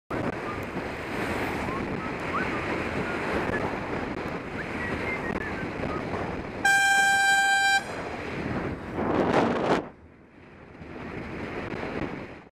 AirZound comes in handy and stops a driver (WM12CZD)
Probably the best use of my AirZound so far.